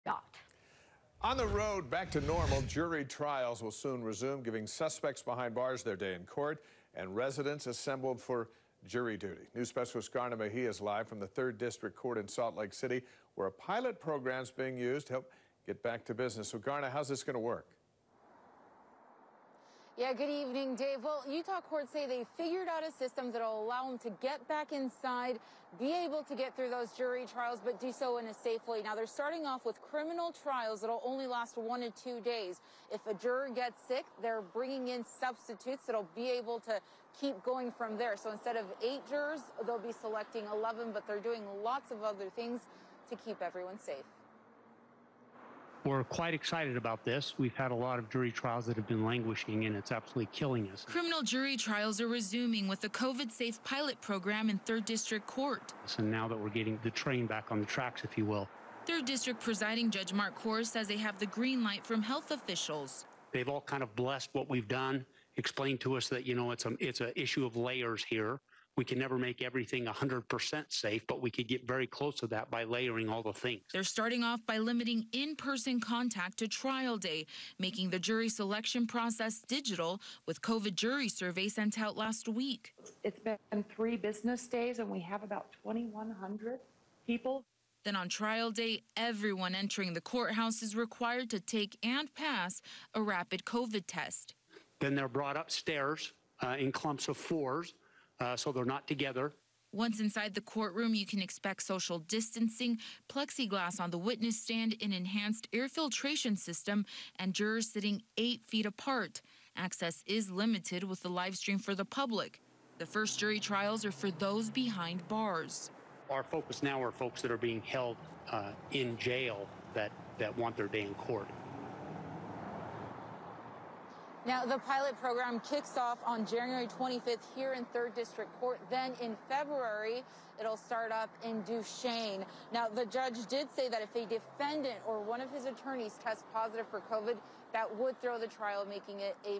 Jury Trials To Resume In Utah After Pandemic Pause
After nearly a year on pause, jury trials will soon resume, giving suspects behind bars their day in court and residents assembled for jury duty.